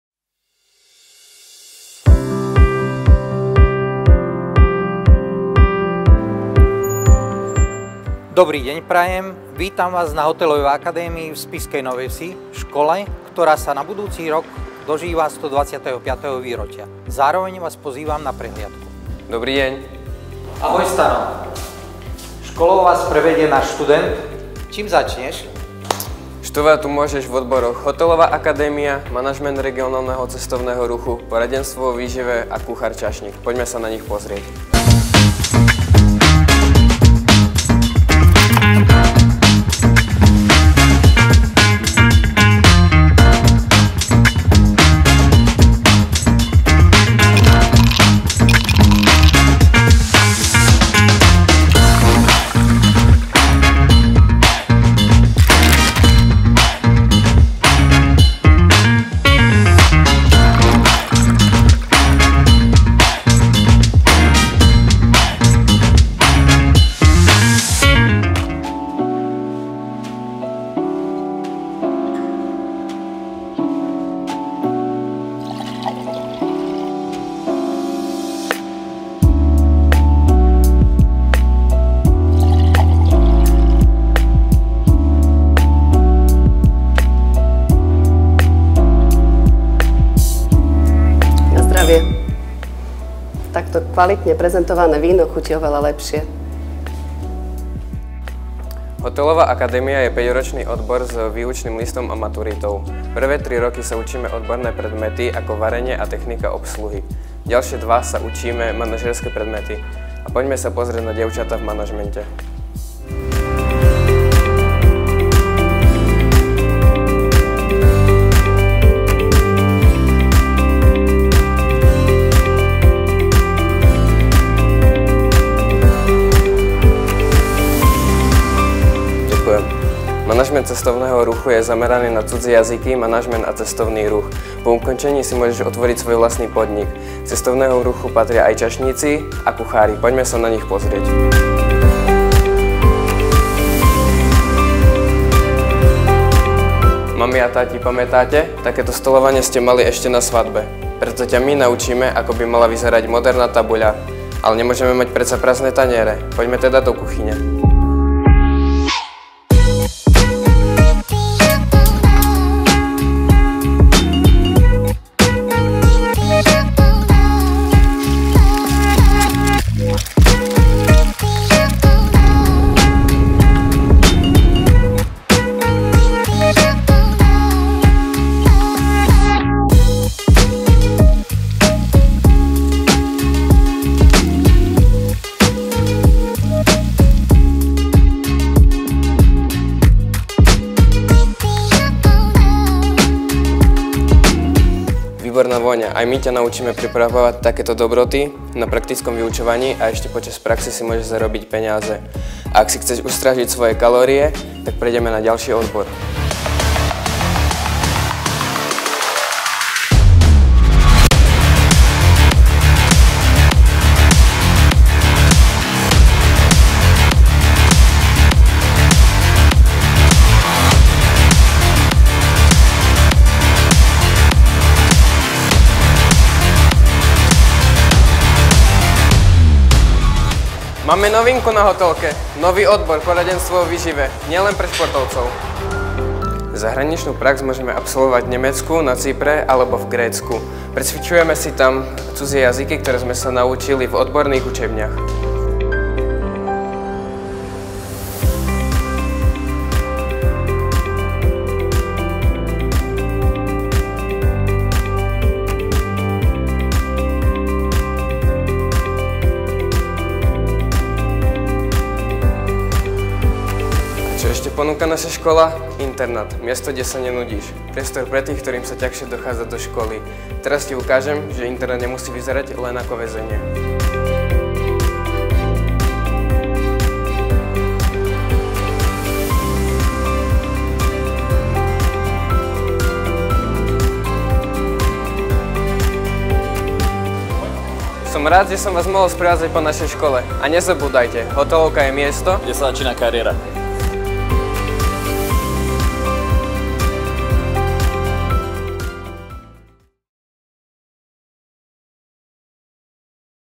Hotelová akadémia Spišská Nová Ves PROMO VIDEO 2021
Deň otvorených dverí na našej škole v piatich minútach.